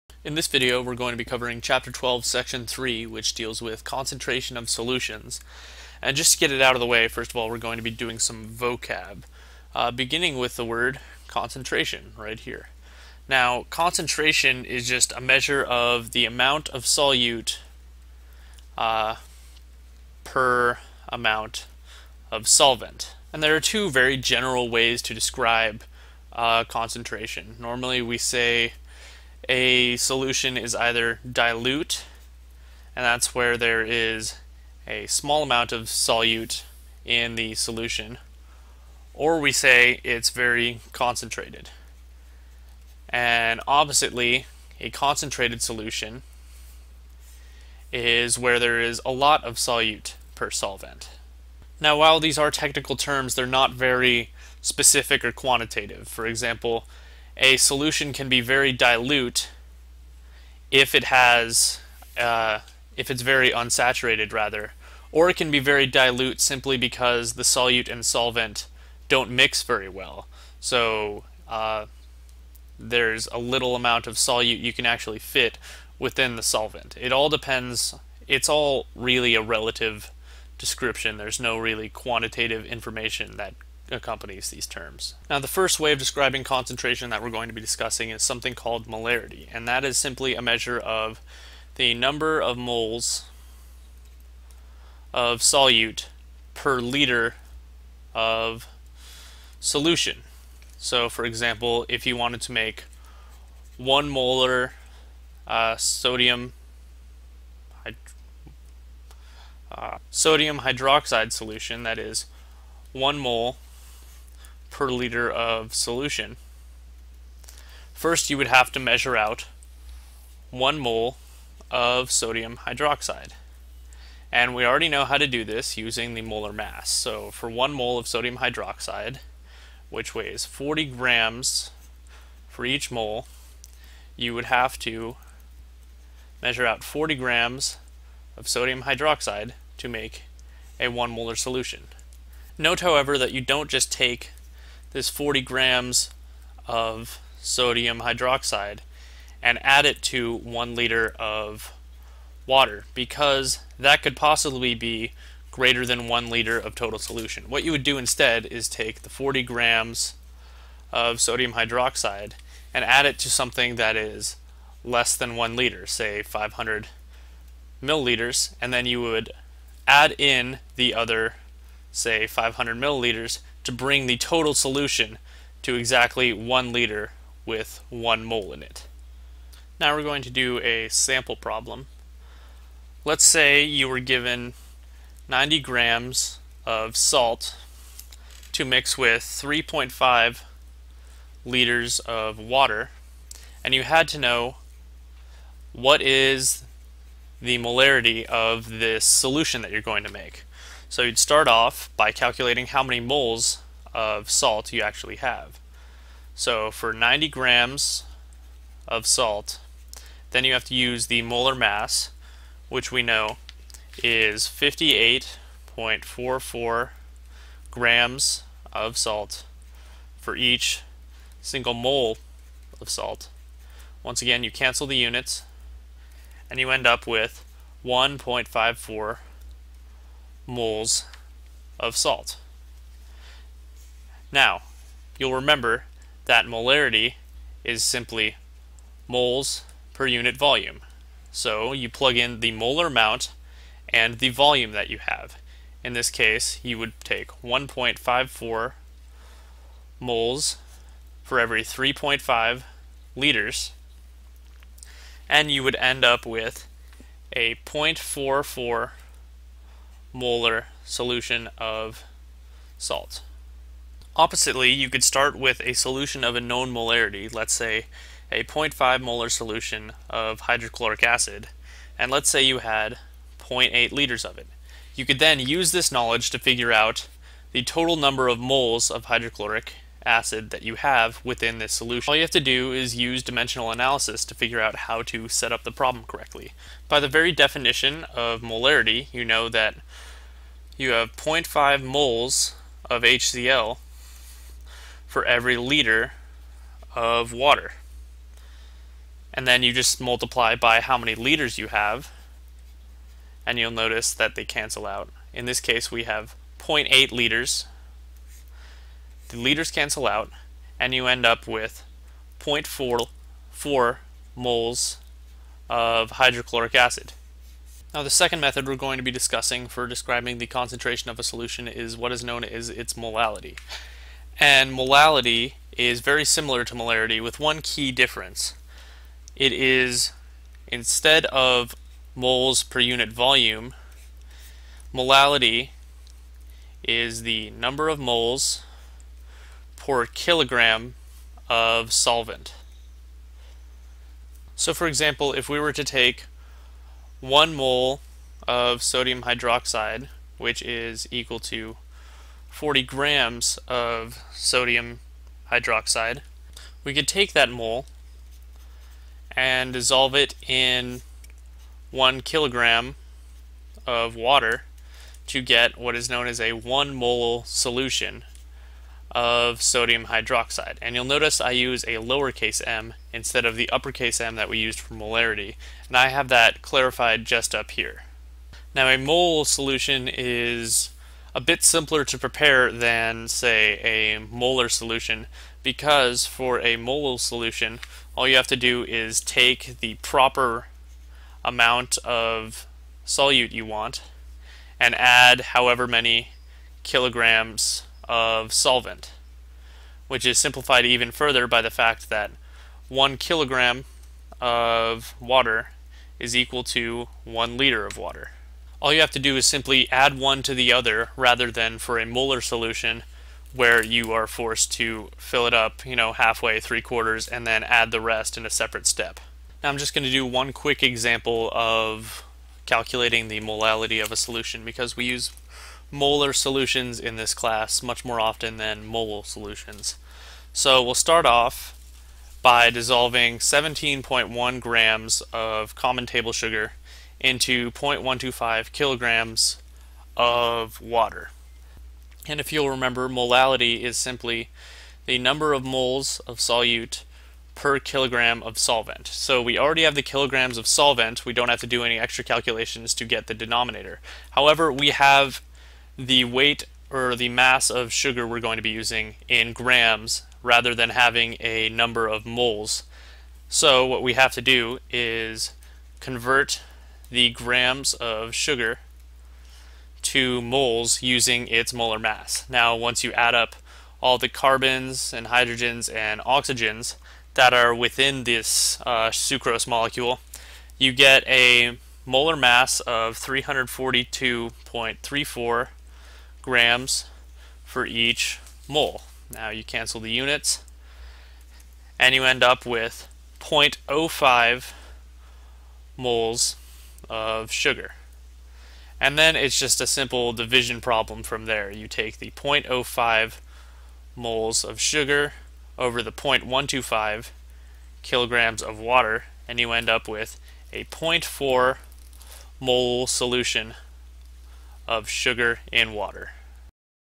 12.3 Concentration of Solutions
This video discusses concentration, including measurement of molarity and molality. Sample problems are shown for calculating both values.